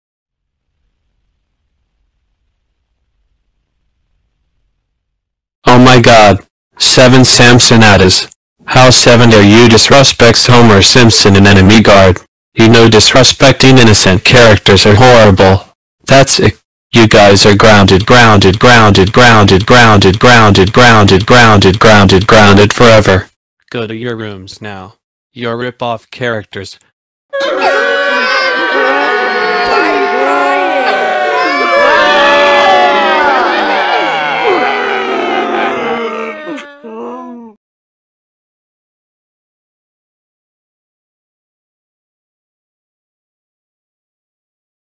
7 Samsonadzes gets Grounded by Miles "Tails" Prower for Disrespecting Homer Simpson & Enemy Guard
Not Credit to Windows Polish, Simo & Mauro, Antonin S VGCP, Diego Requenez, American Standard Madera UTTP THDTC TSPL, Diego Requenez, and Bubble336 Cuba909

Credit to Innocent friends